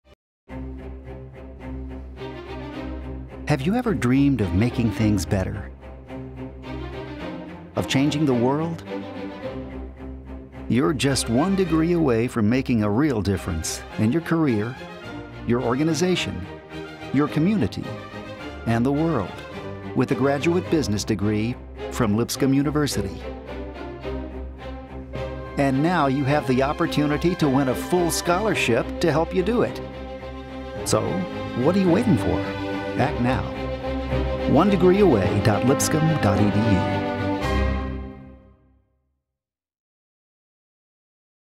Degrees of Difference Scholarship Video Competition
Video responses accepted between March 1, 2013 to May 15, 2013.

You're one degree away from making a difference in the world. And less than 2 minutes away from the scholarship that will help you do it.

Compete to win a FULL SCHOLARSHIP for Fall 2013 to the Lipscomb graduate business degree program of your choice:

MBA • PMBA • MAcc • MHR

It's easy. Share your story. Post your video. One worthy individual committed to making their organization, community or world a better place will be awarded a FULL SCHOLARSHIP this fall.  Why shouldn't it be you?

Video comments will be posted pending approval.  Voting begins June 10, 2013.